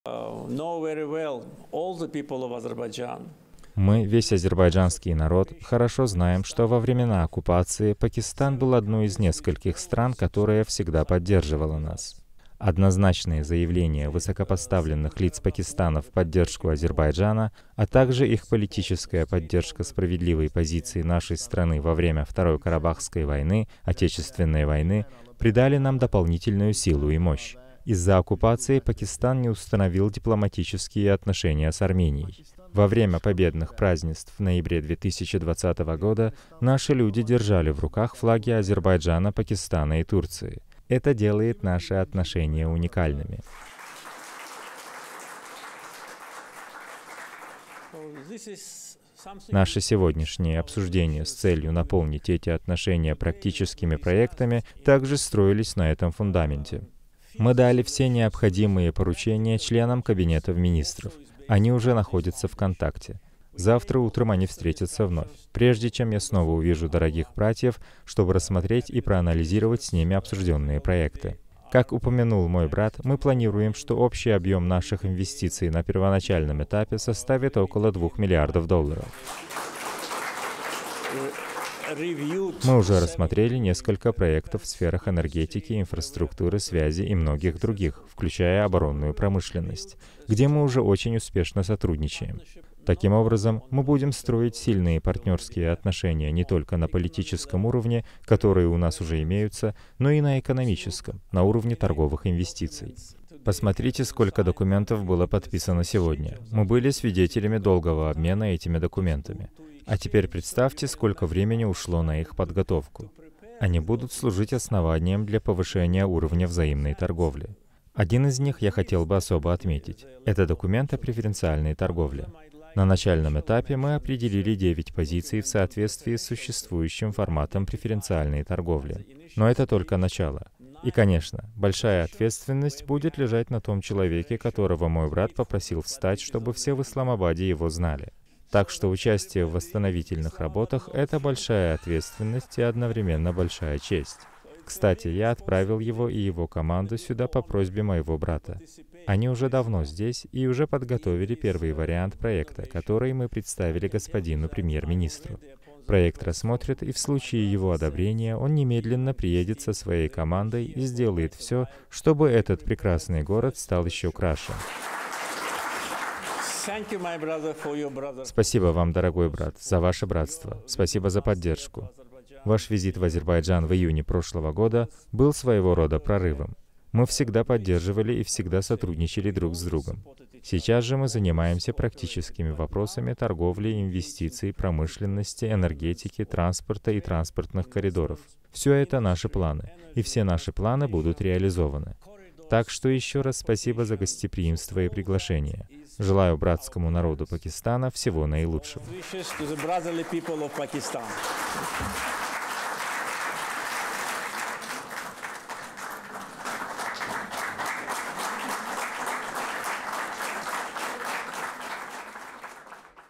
Президент Ильхам Алиев о подписанных с Пакистаном документах
11 июля Президент Азербайджанской Республики Ильхам Алиев и премьер-министр Исламской Республики Пакистан Мухаммад Шахбаз Шариф выступили в Исламабаде с заявлениями для прессы.